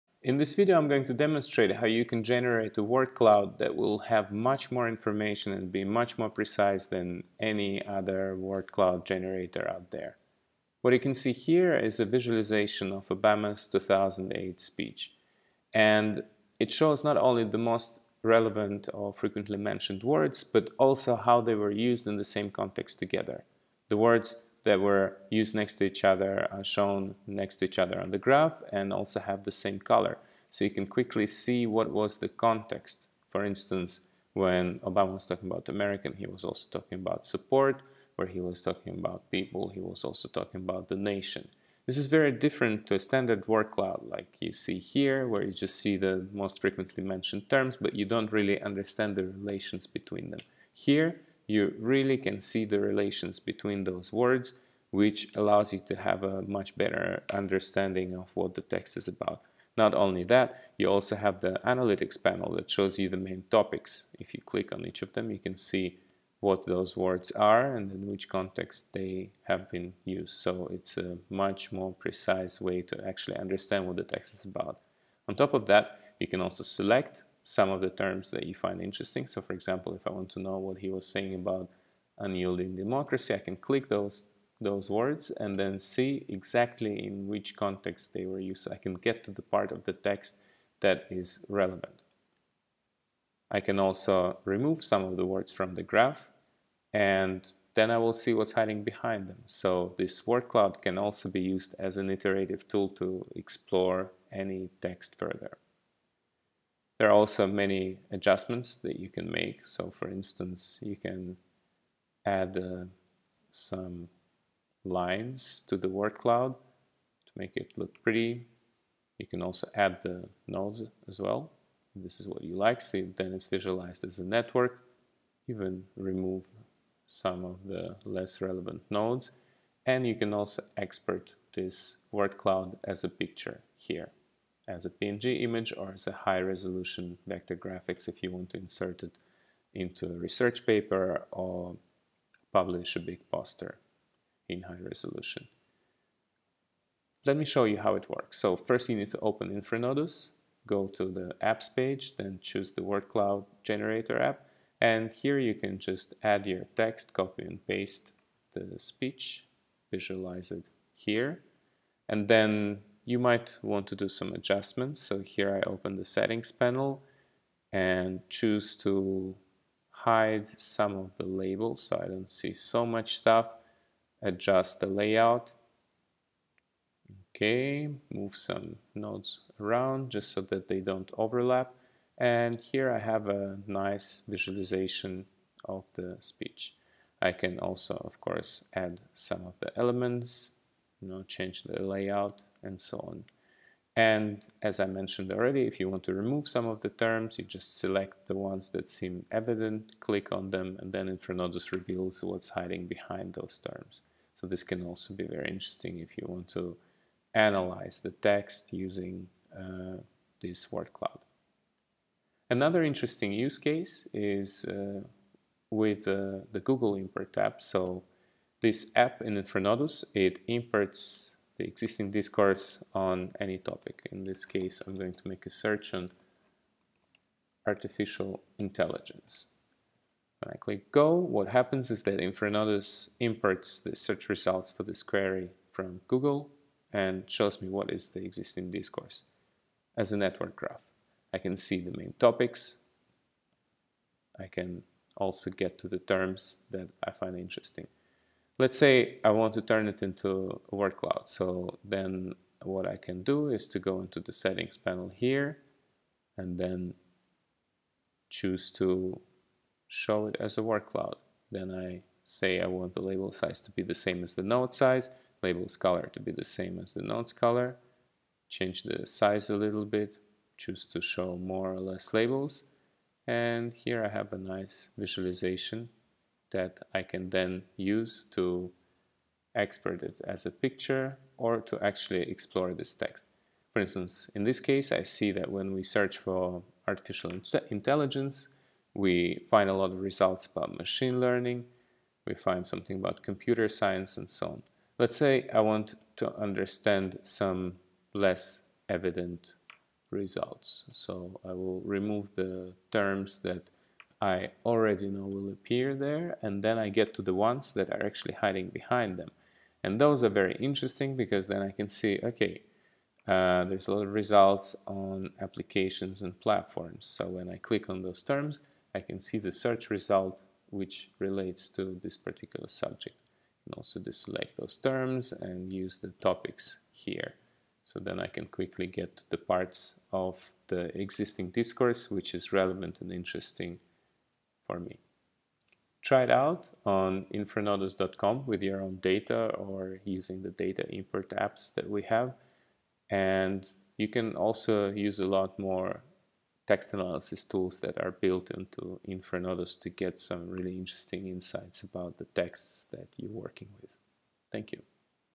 Generate a Word Cloud: Visualize and Analyze any Text or Discourse
Unlike standard word cloud generators, InfraNodus shows not only the most frequent words but also the relations between them. So you can retain the context and get to the parts of the text that are relevant for you. You can use the word cloud to manipulate your text and derive some additional insights by removing the most popular terms from the visualization to get to the more specific parts of the text.

As InfraNodus is based on text network analysis, you have a powerful analytics panel that shows the main topical clusters present within the text, so you can get a much more precise understanding of what the text is actually talking about.

A similar approach can be used to study an existing discourse, as we show in this video using the example of Google search results for "artificial intelligence".